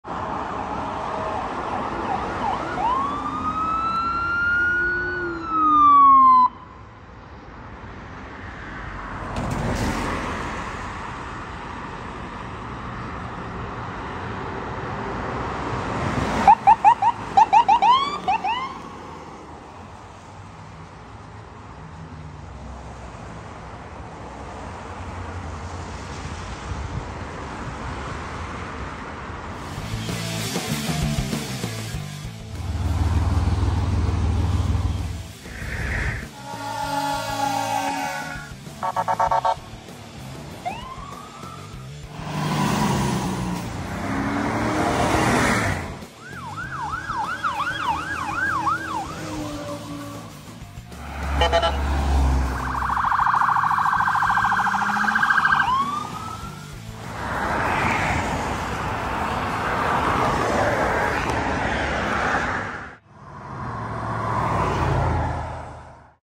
Englewood Health EMS responding 5-4-23
Englewood Health EMS transporting emergent to Englewood Hospital. unfortunately missed them responding to the call